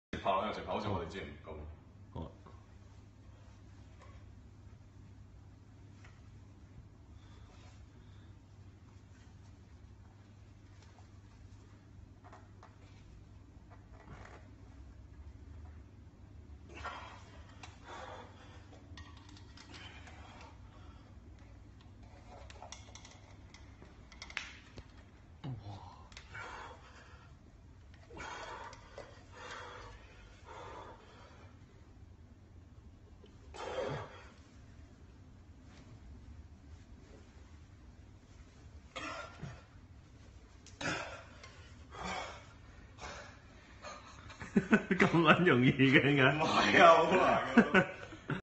Stainless Steel Frying Pan Roll (Feat of Strength Work)
Was asked to give the JHC bought frying pan a roll, most pans people do demos with tend to be easily rolled this one is pretty tough. As with all forms of steel bending once you push through and get the thing moving you want to keep the steel in motion as the steel heats up, if you pause and let it "cool off" it will be much harder to twist the steel into different shape.

Rolling frying pans primarily takes grip and wrist strength to actually roll them up into that shape instead of just folding them by putting weight on it.

Hong Kong Kettlebell Strength & Conditioning Training Group
Hong Kong Old Time Strongman Feats of Strength Interest